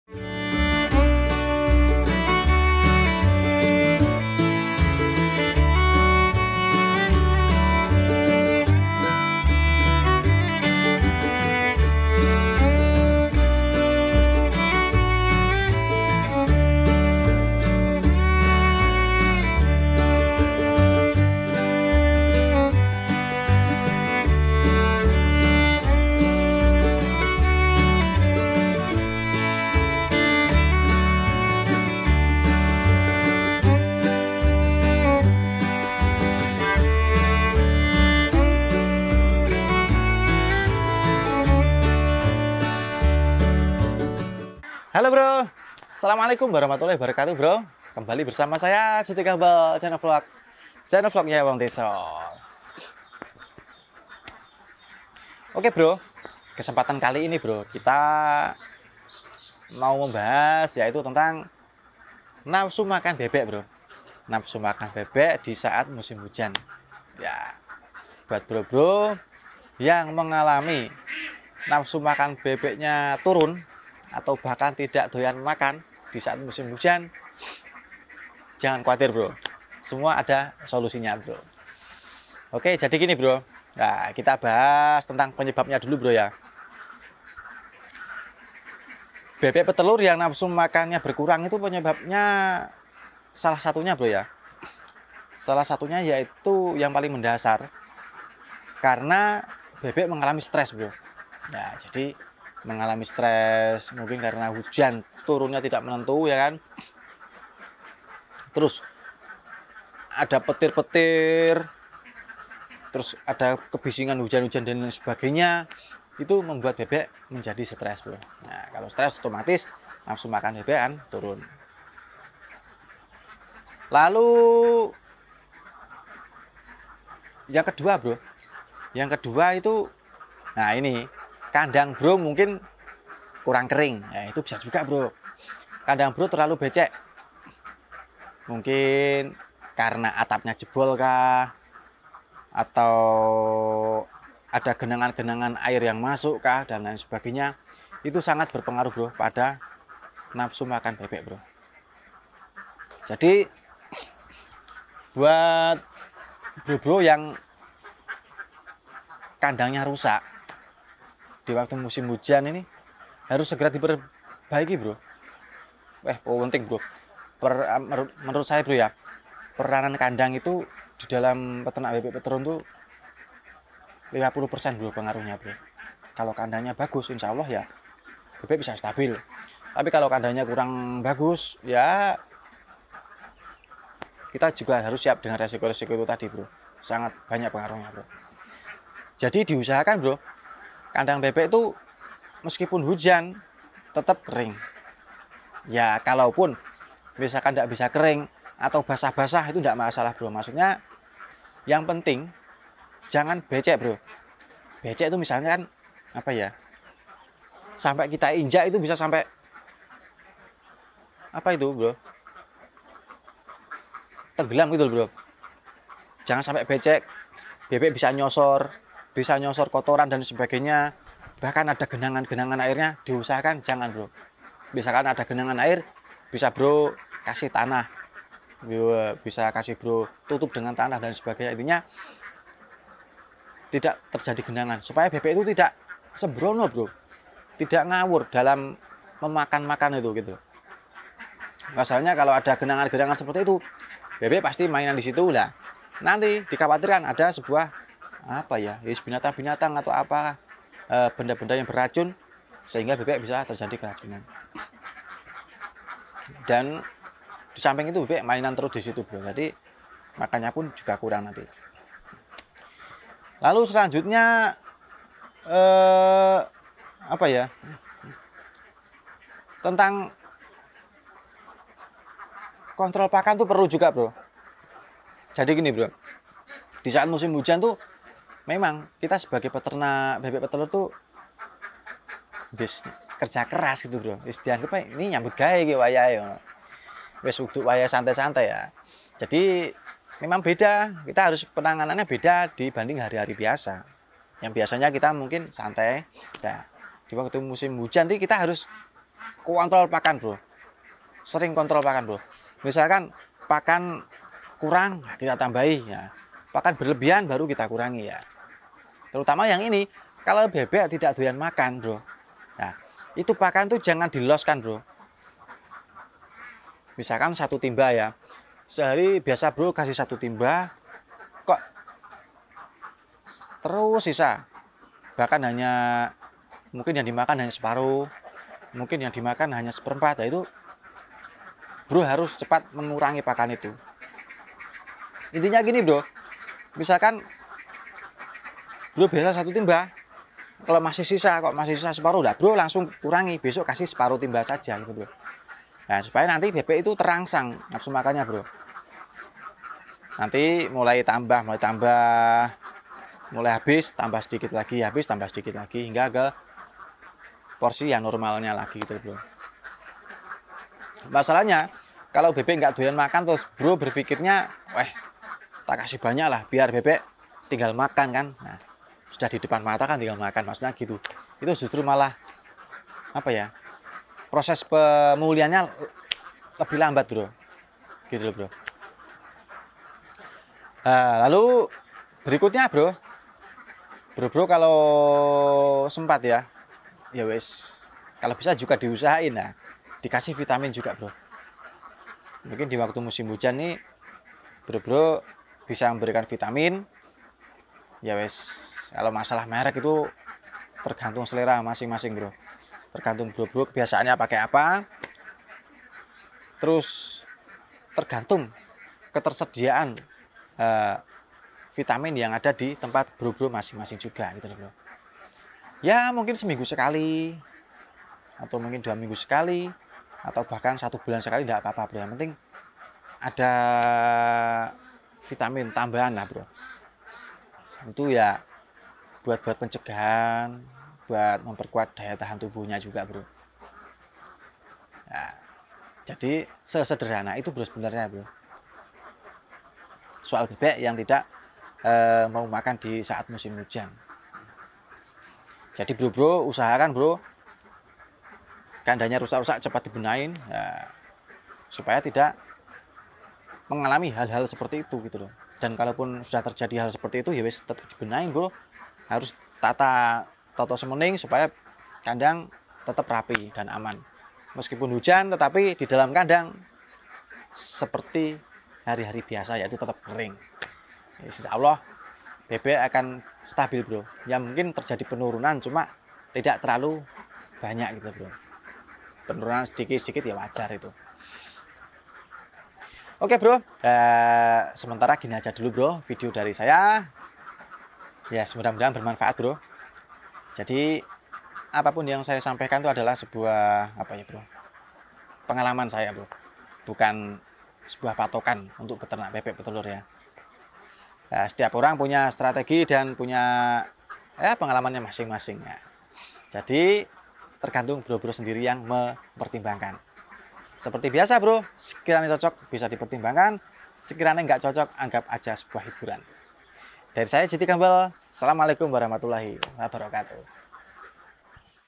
PAKAN SISA BANYAK DAN PRODUKSI TELUR TURUN. SOLUSI BEBEK PETELUR DI MUSIM HUJAN
salam wekwek brow. bebek petelur gak doyan makan di musim hujan. nafsu makan menurun produksi juga turun. banyak faktor penyebab nafsu makan bebek menurun. dan ini cara penanganan nya, dan juga cara pencegahan nya#bebekpetelur#bebekblitar#peternakmuda#peternaksukses#jutikhumble#vlog#